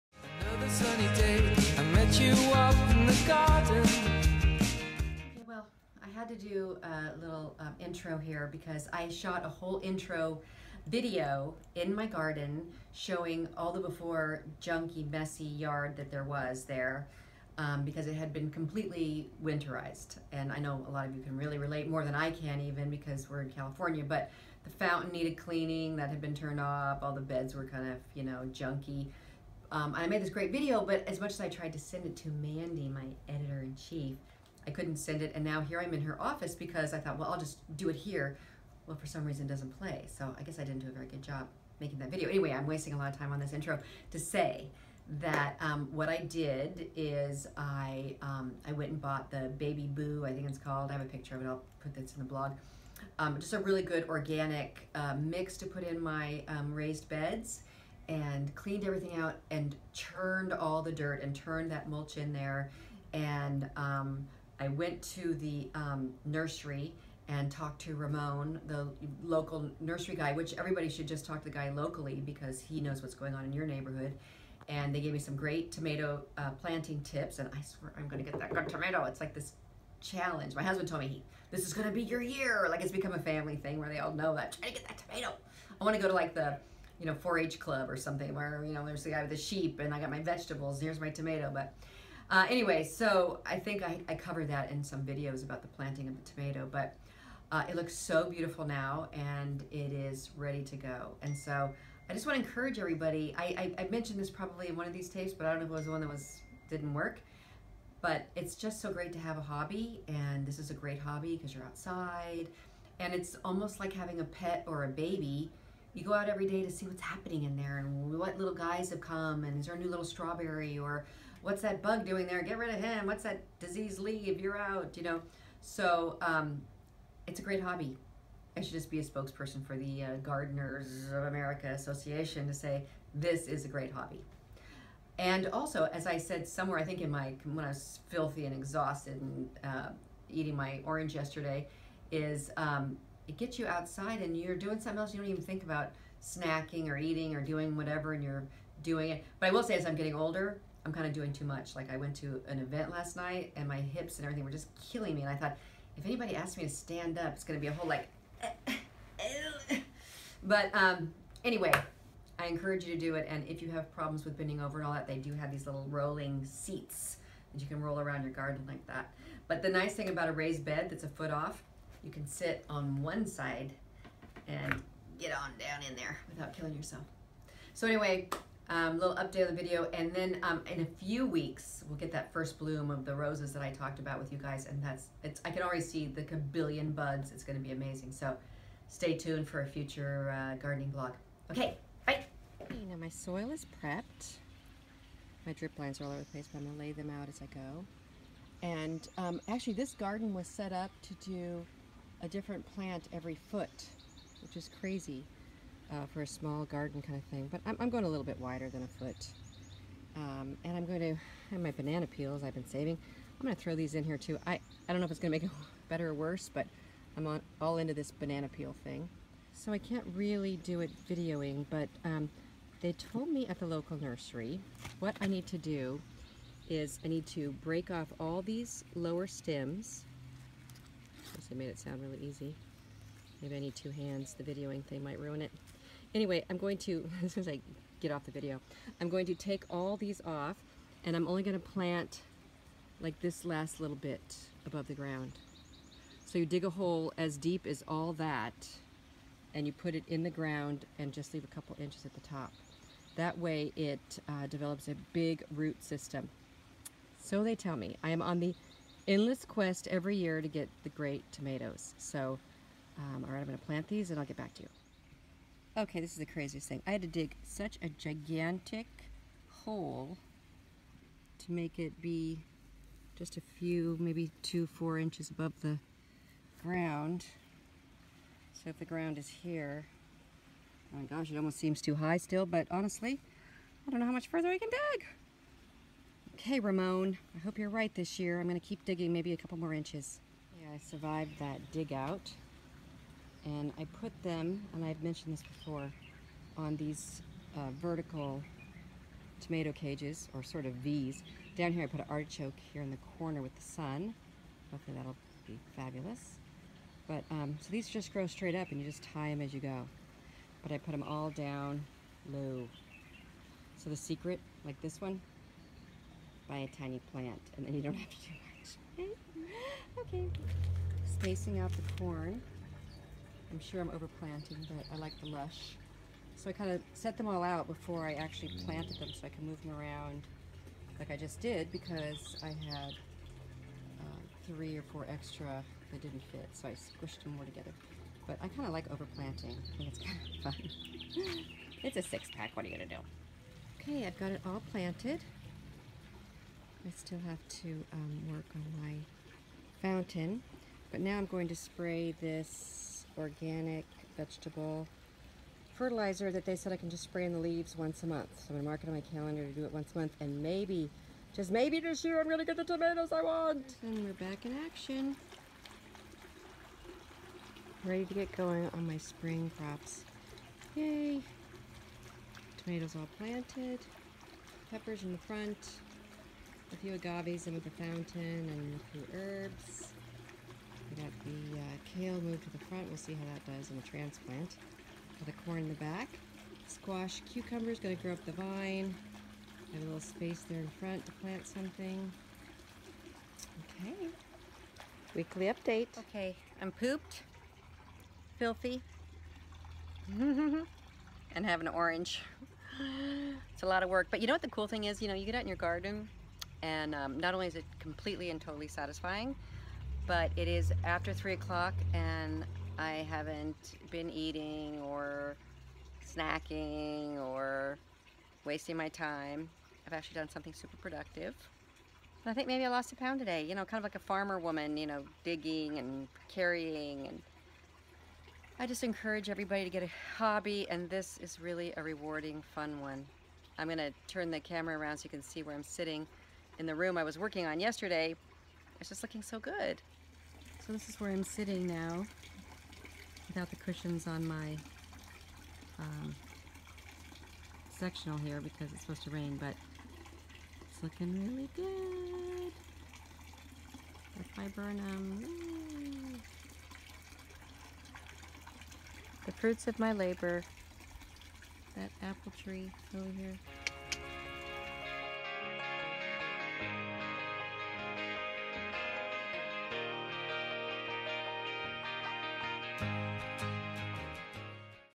My Spring Garden
Mary Beth Evans shares her Spring gardening work with the readers of her blog, PLANK, including her attempt to grow a great tomato.